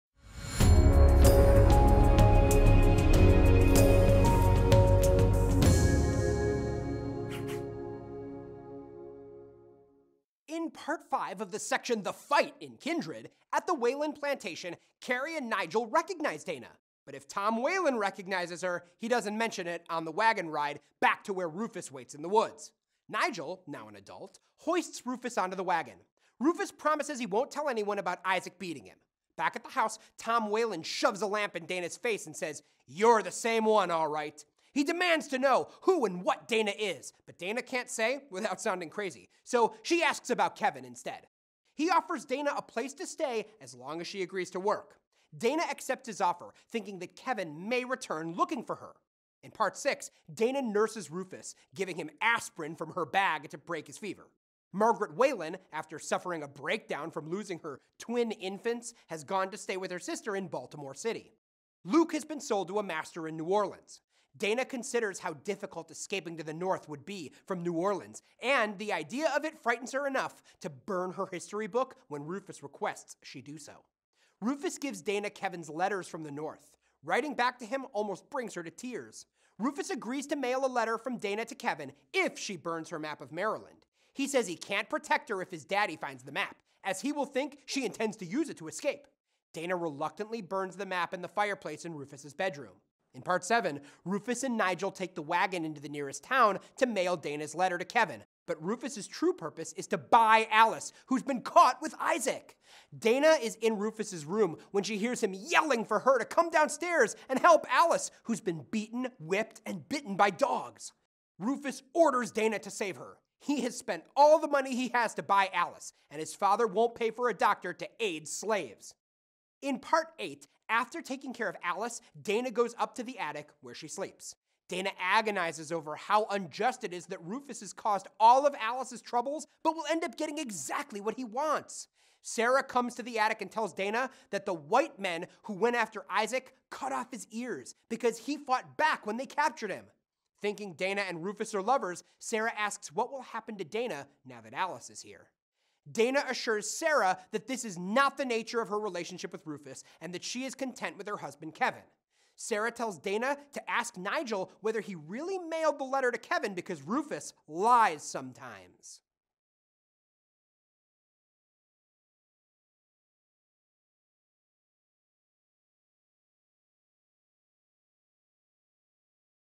Kindred by Octavia Butler | The Fight, Parts 5–8
Summarize videos instantly with our Course Assistant plugin, and enjoy AI-generated quizzes Octavia Butler’s Kindred explained with part summaries in just a few minutes!

Course Hero Literature Instructor Russell Jaffe provides an in-depth summary and analysis of The Fight, Parts 5–8 of Octavia Butler's novel Kindred.

Octavia Butler’s Kindred tells the story of Dana, a black woman from the 1970s who time travels to a 19th-century slave plantation by unexplained forces each time her ancestor Rufus is in danger.

The son of a white slave owner, Rufus is her great-grandfather. Her jumps in time ensure that he in fact fathers her grandmother, despite the violence that this entails. On each trip, the choices she makes to stay alive become more difficult.

In Kindred, sci-fi author Octavia Butler uses the conventions of her chosen genre to illustrate the generational conflicts defining contemporary black America.

The genealogy of many, if not most, black Americans at some point merges with that of white Americans due to this tortured history. The sexual relationships—consensual or not—between slave owners and their slaves complicate black American history.

Using the conceits of science fiction, the story explores the genesis of contemporary African American life. Through a fantastical but internally plausible narrative, Butler creates a disturbing and beautiful portrait of American history.

African American science fiction writer Octavia Butler’s Kindred was first published in 1979. Published three years after her first novel, Kindred was a breakout success. Butler's masterful writing won her two Hugo Awards (1984 and 1985), science fiction's highest honor.

The science fiction novel Kindred contains many powerful motifs, including slavery, as Dana, a modern woman, bears witness to and endures the horrors of slavery; time travel, as Dana experiences slavery through the recurrent element of time travel; and self-inflicted wounds, as Dana cuts herself in order to time travel home, just as slaves sometimes kill themselves to escape bondage. Important themes include power, kinship, and violence.